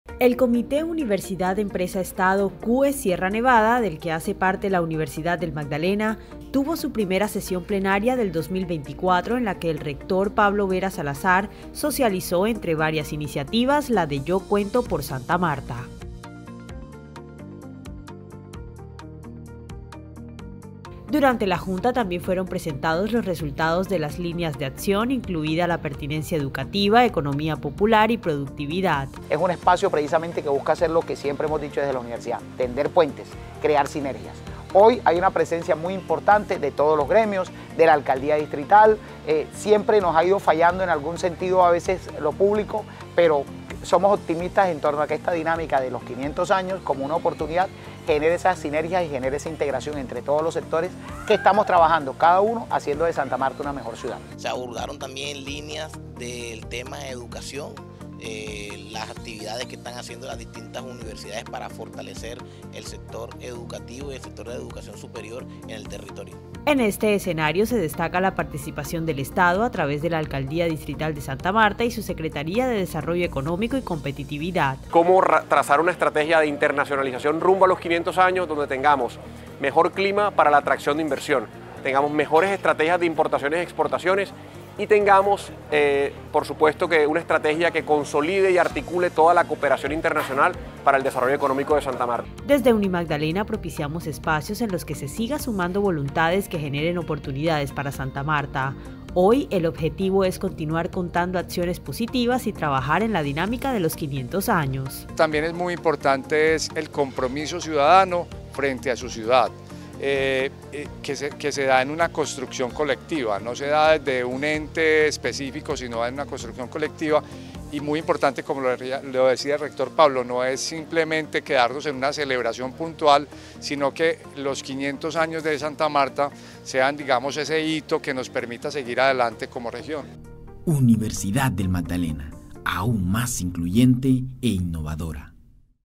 En el CUEE Sierra Nevada también se trabaja en pro de la Santa Marta de los 500 años
Con la presencia de diferentes representantes de gremios, empresarios y la Secretaría de Desarrollo Económico y Competitividad del Distrito de Santa Marta se dio cumplimiento a la sesión plenaria del Comité Universidad Empresa Estado – CUEE Sierra Nevada, donde la Universidad del Magdalena ejerce la Secretaría Técnica y en la que se conocieron los resultados de sus líneas de acción.